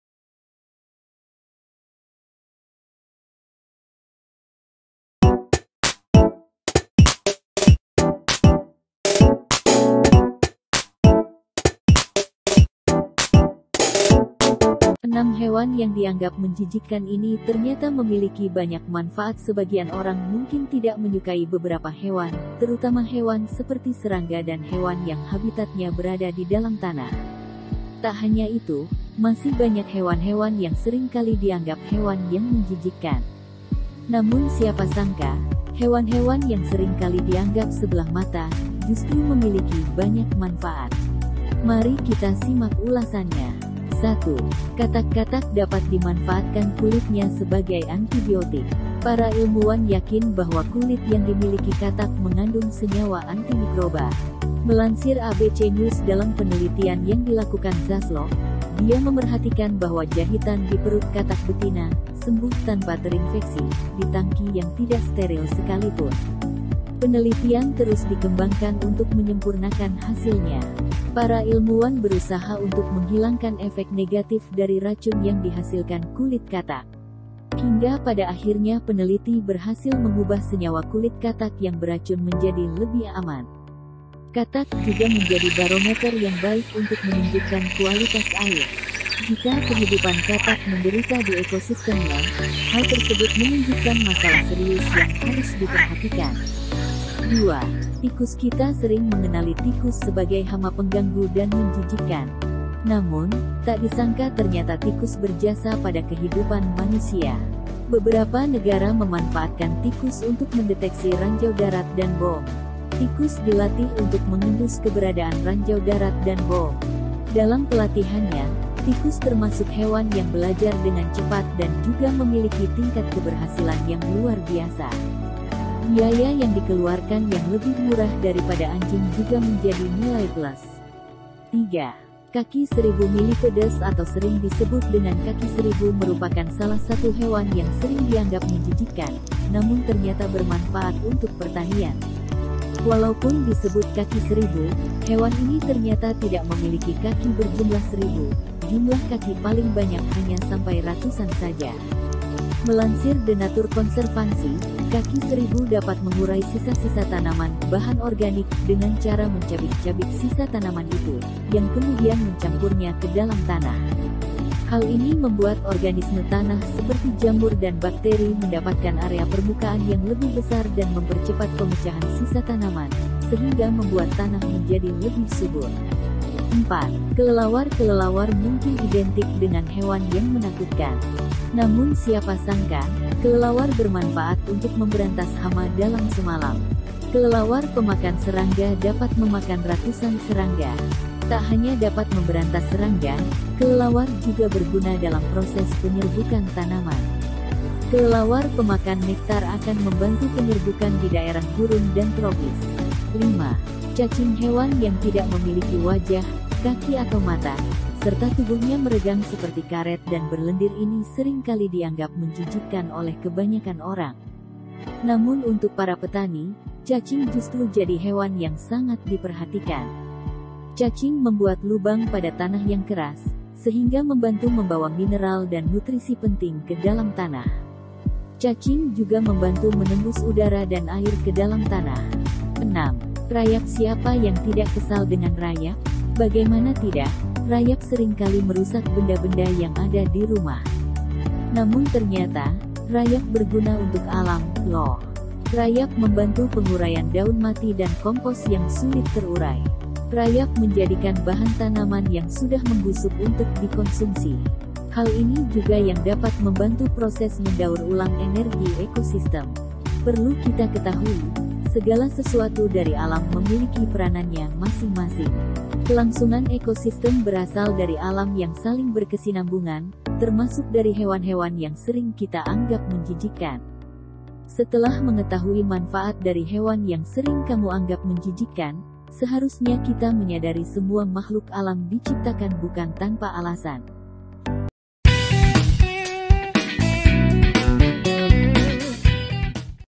6 Hewan yang Dianggap Menjijikkan ini Ternyata Memiliki Banyak Manfaat
Sebagian orang mungkin tidak menyukai beberapa hewan, terutama hewan seperti serangga dan hewan yang habitatnya berada di dalam tanah. Tak hanya itu, masih banyak hewan-hewan yang sering kali dianggap hewan yang menjijikkan.
Namun siapa sangka, hewan-hewan yang sering kali dianggap sebelah mata, justru memiliki banyak manfaat. Mari kita simak ulasannya.
1. Katak
dapat dimanfaatkan kulitnya sebagai antibiotik. Para ilmuwan yakin bahwa kulit yang dimiliki katak mengandung senyawa antimikroba. Melansir ABC News dalam penelitian yang dilakukan Zasloff, dia memerhatikan bahwa jahitan di perut katak betina, sembuh tanpa terinfeksi, di tangki yang tidak steril sekalipun.
Penelitian terus dikembangkan untuk menyempurnakan hasilnya. Para ilmuwan berusaha untuk menghilangkan efek negatif dari racun yang dihasilkan kulit katak. Hingga pada akhirnya peneliti berhasil mengubah senyawa kulit katak yang beracun menjadi lebih aman.
Katak juga menjadi barometer yang baik untuk menunjukkan kualitas air. Jika kehidupan katak menderita di ekosistemnya, hal tersebut menunjukkan masalah serius yang harus diperhatikan.
2. Tikus
Kita sering mengenali tikus sebagai hama pengganggu dan menjijikkan. Namun, tak disangka ternyata tikus berjasa pada kehidupan manusia.
Beberapa negara memanfaatkan tikus untuk mendeteksi ranjau darat dan bom. Tikus dilatih untuk mengendus keberadaan ranjau darat dan bom. Dalam pelatihannya, tikus termasuk hewan yang belajar dengan cepat dan juga memiliki tingkat keberhasilan yang luar biasa. Biaya yang dikeluarkan yang lebih murah daripada anjing juga menjadi nilai plus.
3. Kaki seribu
Millipedes atau sering disebut dengan kaki seribu merupakan salah satu hewan yang sering dianggap menjijikkan, namun ternyata bermanfaat untuk pertanian. Walaupun disebut kaki seribu, hewan ini ternyata tidak memiliki kaki berjumlah seribu, jumlah kaki paling banyak hanya sampai ratusan saja.
Melansir The Nature Conservancy, kaki seribu dapat mengurai sisa-sisa tanaman (bahan organik) dengan cara mencabik-cabik sisa tanaman itu, yang kemudian mencampurnya ke dalam tanah. Hal ini membuat organisme tanah seperti jamur dan bakteri mendapatkan area permukaan yang lebih besar dan mempercepat pemecahan sisa tanaman, sehingga membuat tanah menjadi lebih subur.
4. Kelelawar
mungkin identik dengan hewan yang menakutkan. Namun siapa sangka, kelelawar bermanfaat untuk memberantas hama dalam semalam. Kelelawar pemakan serangga dapat memakan ratusan serangga.
Tak hanya dapat memberantas serangga, kelelawar juga berguna dalam proses penyerbukan tanaman. Kelelawar pemakan nektar akan membantu penyerbukan di daerah gurun dan tropis.
5. Cacing
Hewan yang tidak memiliki wajah, kaki atau mata, serta tubuhnya meregang seperti karet dan berlendir ini sering kali dianggap menjijikkan oleh kebanyakan orang. Namun untuk para petani, cacing justru jadi hewan yang sangat diperhatikan.
Cacing membuat lubang pada tanah yang keras, sehingga membantu membawa mineral dan nutrisi penting ke dalam tanah. Cacing juga membantu menembus udara dan air ke dalam tanah.
6. Rayap
Siapa yang tidak kesal dengan rayap? Bagaimana tidak, rayap sering kali merusak benda-benda yang ada di rumah. Namun ternyata, rayap berguna untuk alam. Rayap membantu penguraian daun mati dan kompos yang sulit terurai. Rayap menjadikan bahan tanaman yang sudah membusuk untuk dikonsumsi. Hal ini juga yang dapat membantu proses mendaur ulang energi ekosistem.
Perlu kita ketahui, segala sesuatu dari alam memiliki peranannya masing-masing. Kelangsungan ekosistem berasal dari alam yang saling berkesinambungan, termasuk dari hewan-hewan yang sering kita anggap menjijikkan. Setelah mengetahui manfaat dari hewan yang sering kamu anggap menjijikkan, seharusnya kita menyadari semua makhluk alam diciptakan bukan tanpa alasan.